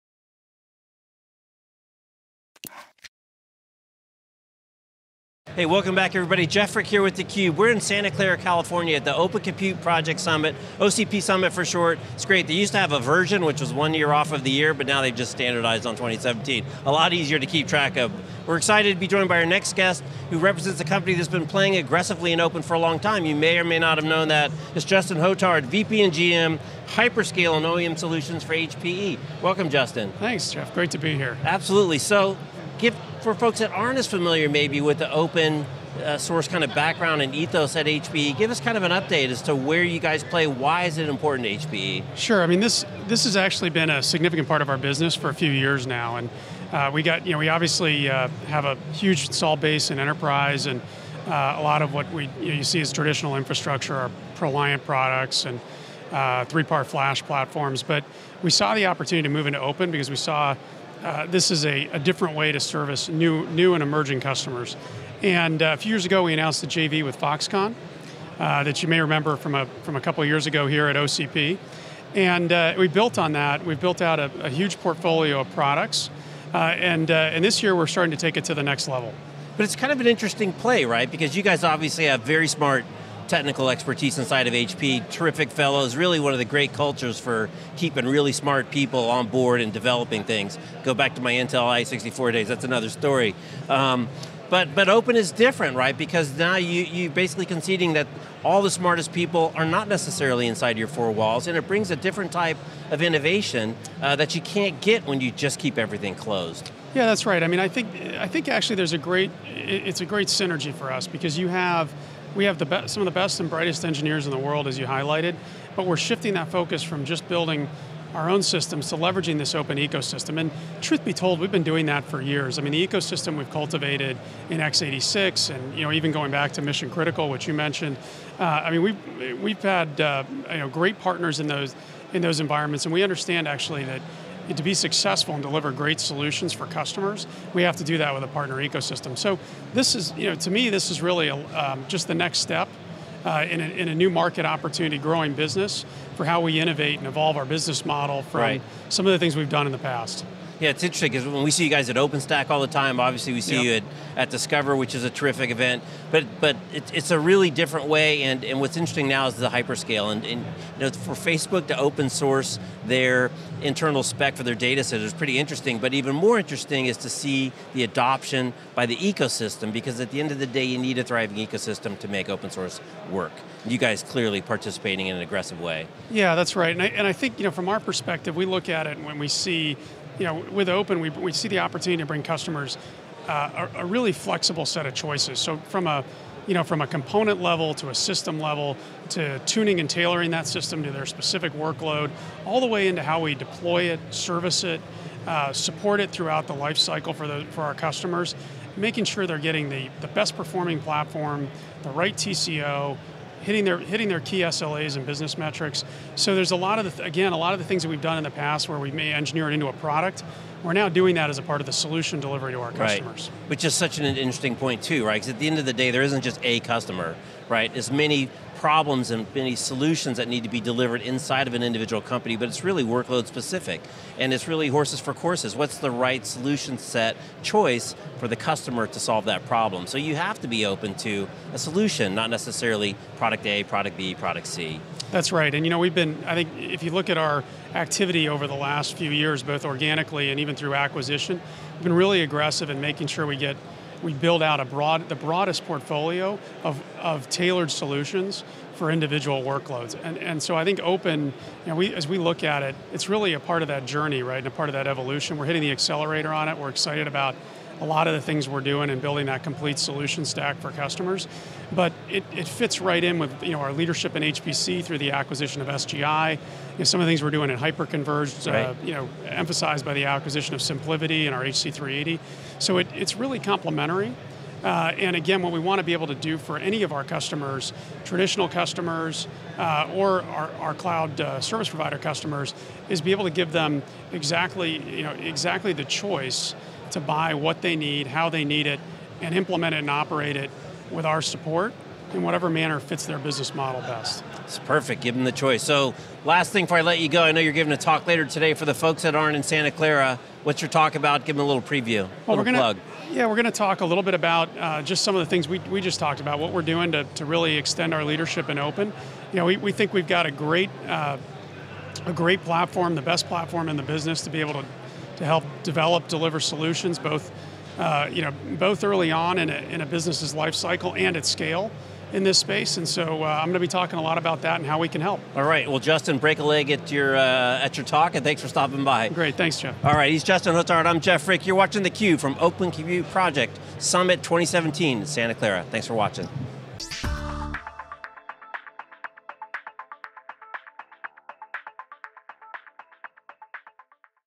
Justin Hotard, HPE | OCP Summit 2017
Justin Hotard, HPE talks with Jeff Frick at OCP Summit 2017 - OCPSummit17